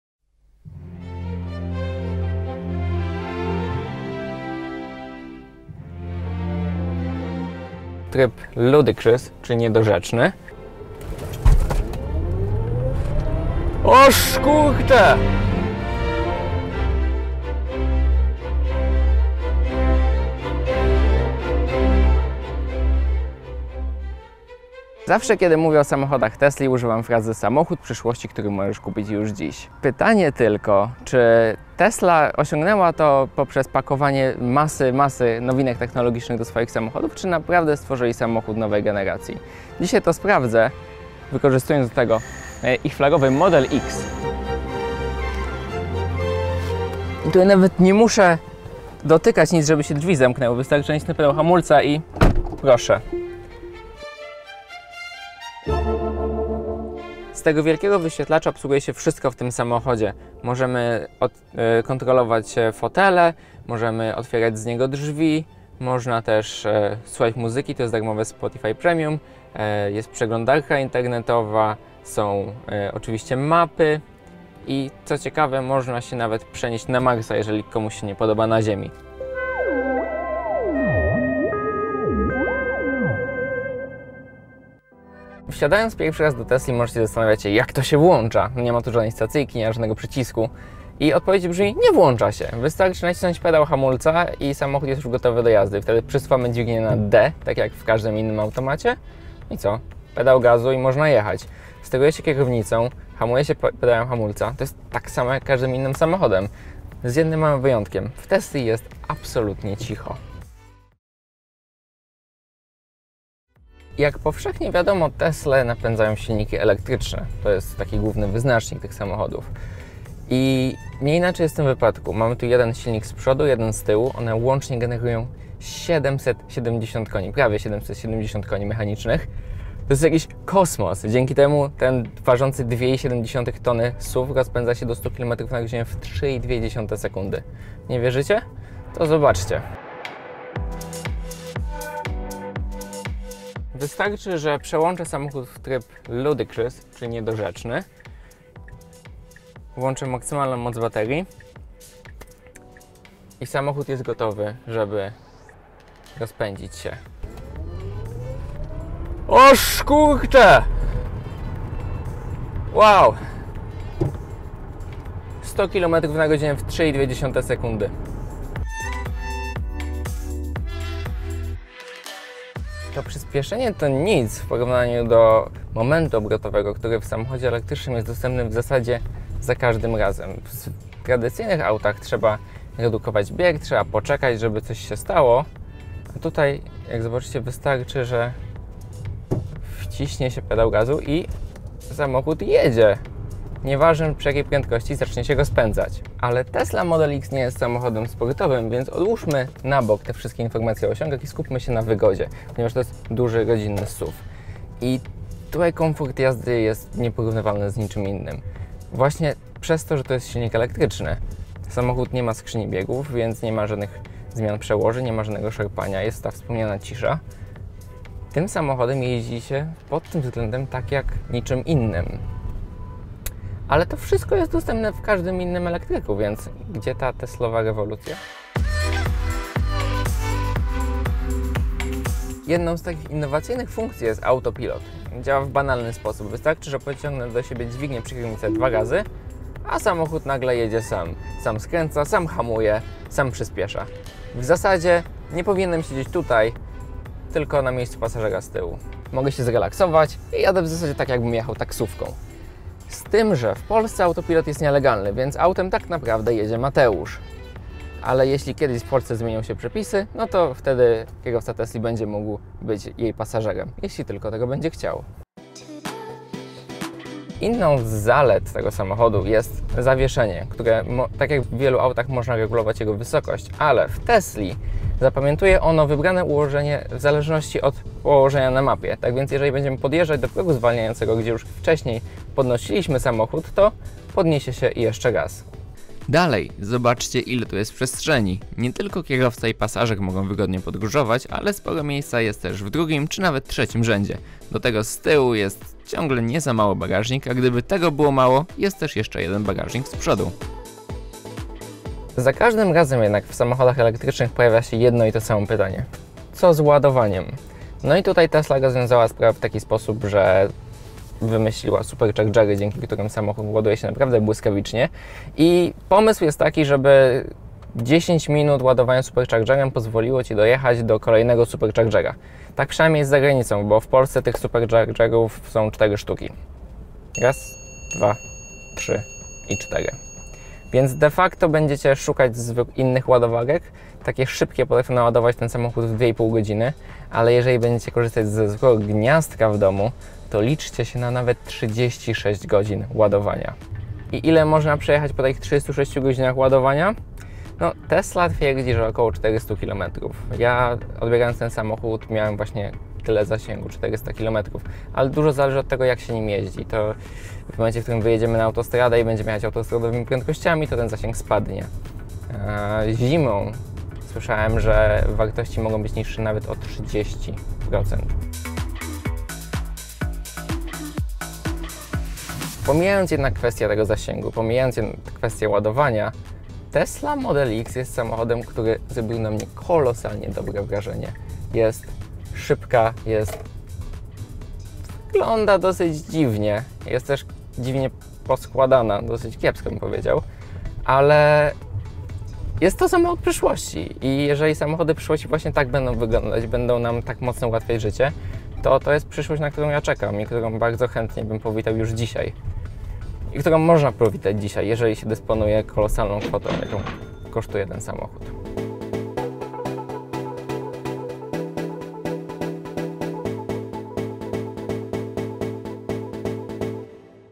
Tesla Model X P90D - Samochód nowej generacji czy gadżet?
Jest wielka, droga i kontrowersyjna. Z jednej strony aż kipi nowoczesnymi rozwiązaniami, z drugiej jest krytykowana za kiepskie wykonanie. Michał Zieliński sprawdził, czy Tesla Model X to naprawdę samochód przyszłości, a może tylko zbędny gadżet.

Dziękujemy TeslaWynajem.pl za użyczenie auta